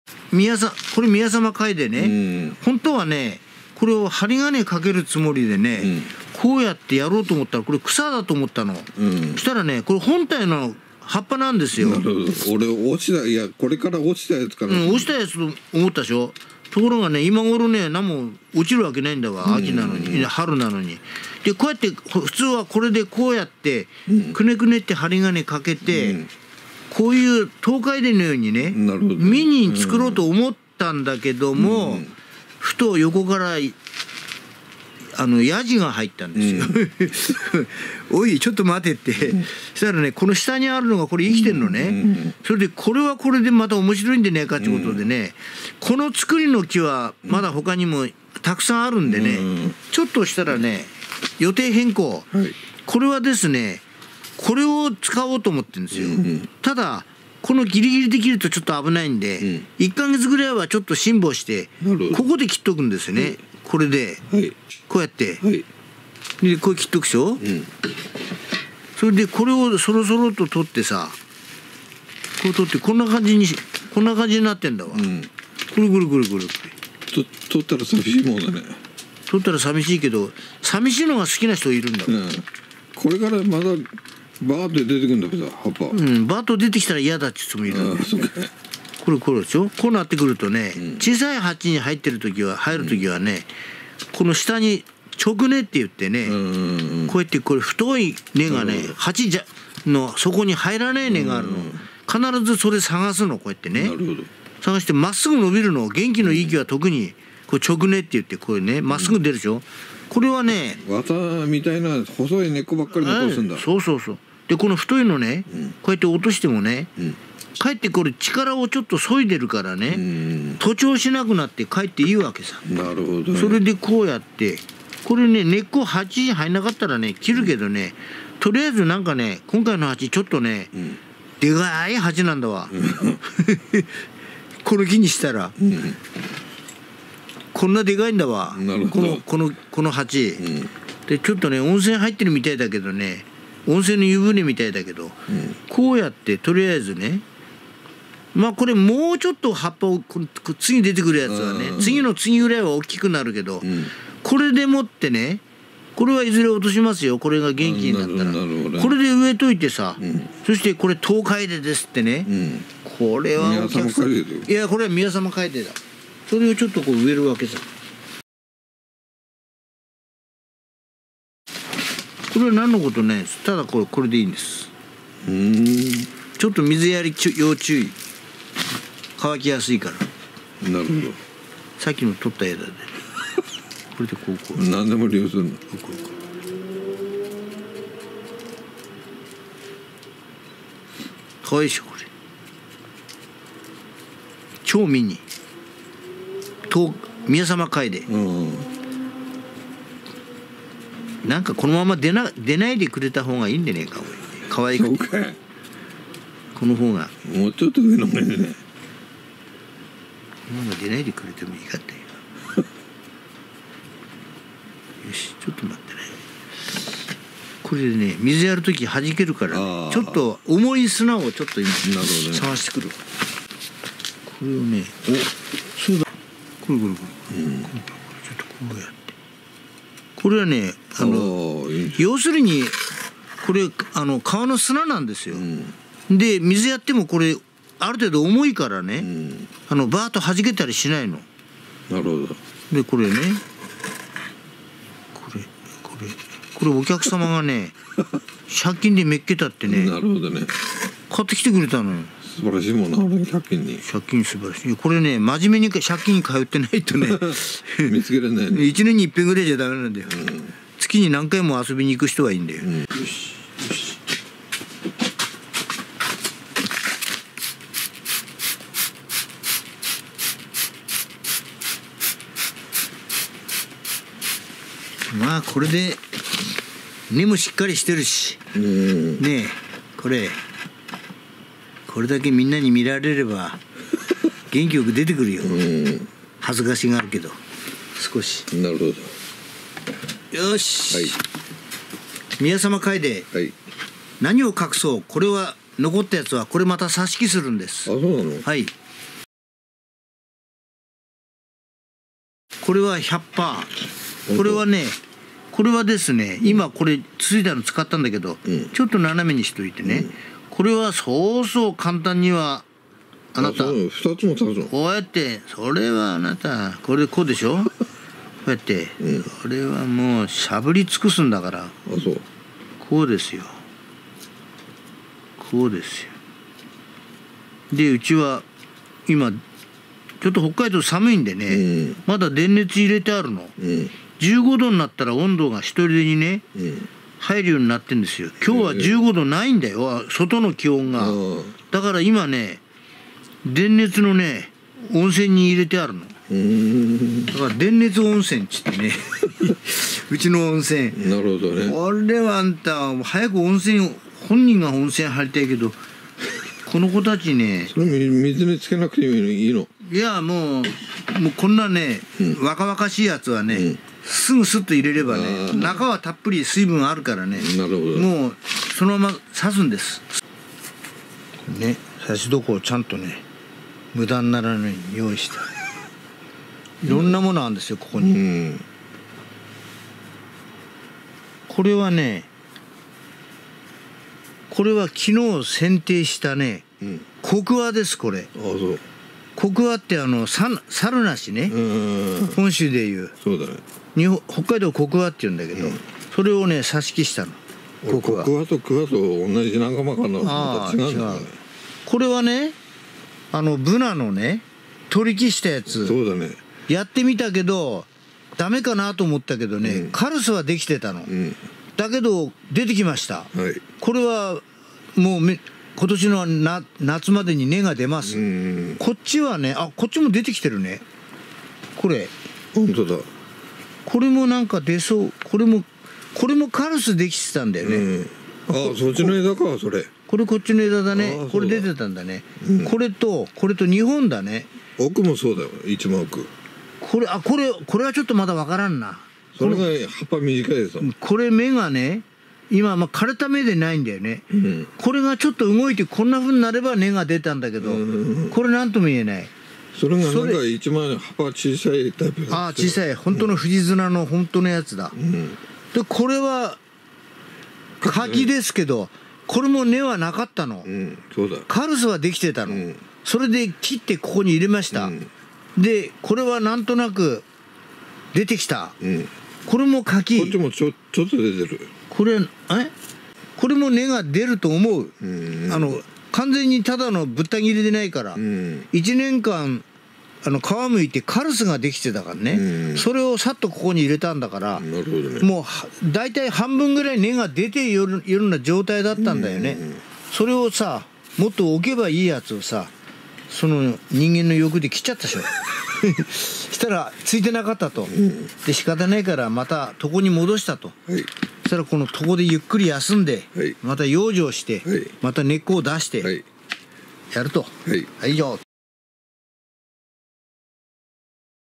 宮様カエデの超ミニ盆栽
宮様カエデの超ミニ盆栽を作るのと挿し木の色々なことを説明しています。